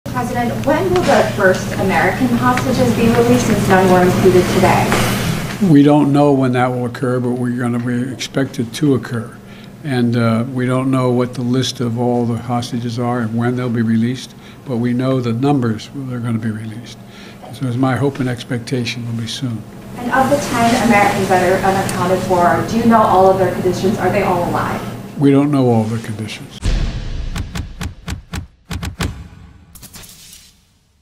Joe Biden Admits Doesn't Know Which Americans Are Held Hostage by Hamas, When They'll Be Released
Doesn't seem like much of a priority, huh Joe? President Biden admitted his administration did not know which Americans are currently held hostage by Hamas, when they will be released, or even if they are still alive.